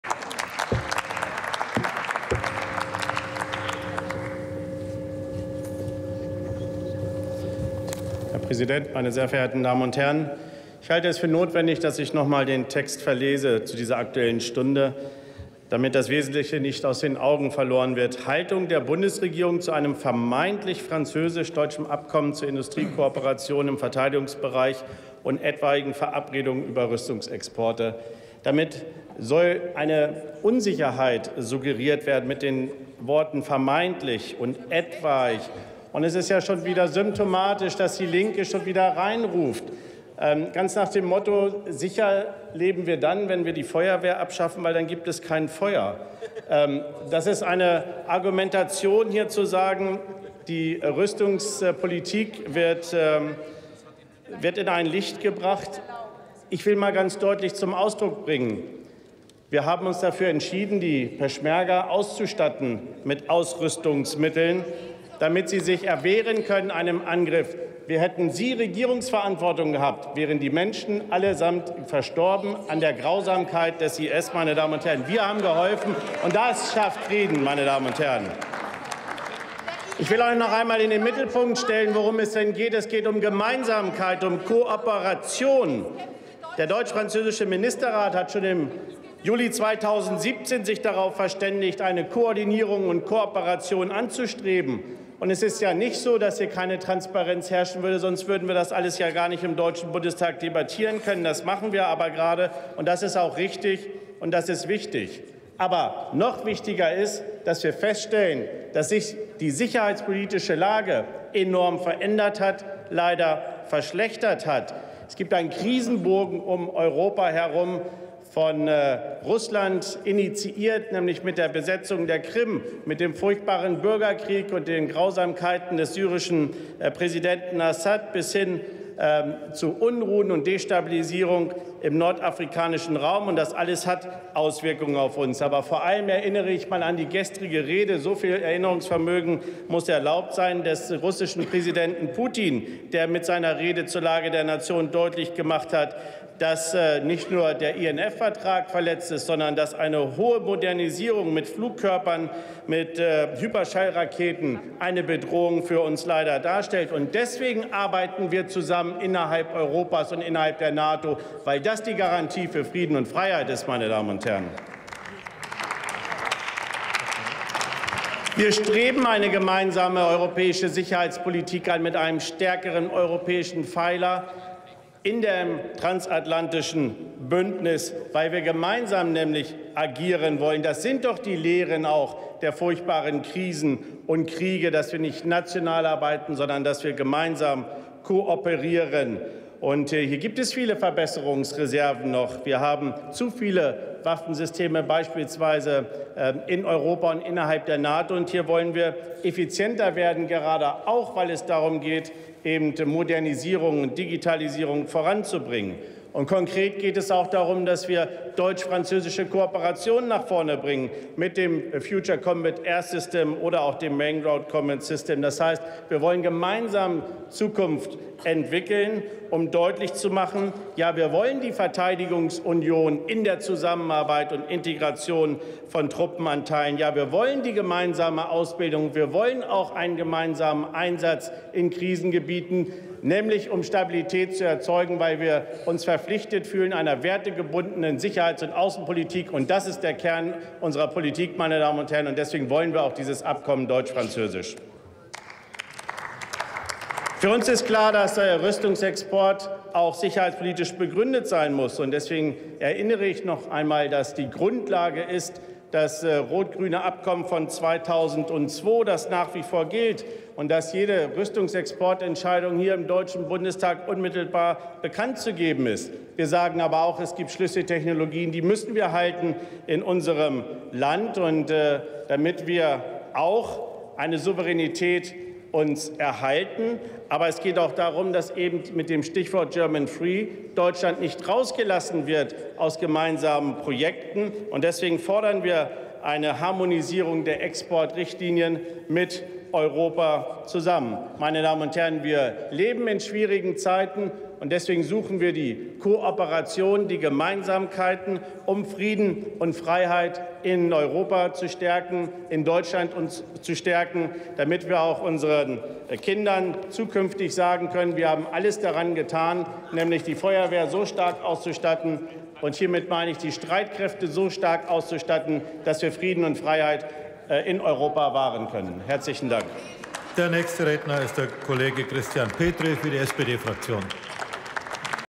Henning Otte: Aktuelle Stunde zu einem vermeintlichen Abkommen mit… [Bundestag 21.02.2019]
Aktuelle Stunde
Haltung der Bundesregierung zu einem vermeintlich französisch-deutschem Abkommen zur Industriekooperation im Verteidigungsbereich und etwaigen Verabredungen über Rüstungsexporte

19. Deutscher Bundestag
83. Plenarsitzung
Tagesordnungspunkt: Aktuelle Stunde zu einem vermeintlichen Abkommen mit Frankreich im Verteidigungsbereich
Redner: Otte, Henning, CDU/CSU
Partei: Christlich Demokratische Union (CDU)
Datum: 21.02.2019
Plenarprotokoll:
UVID:7328895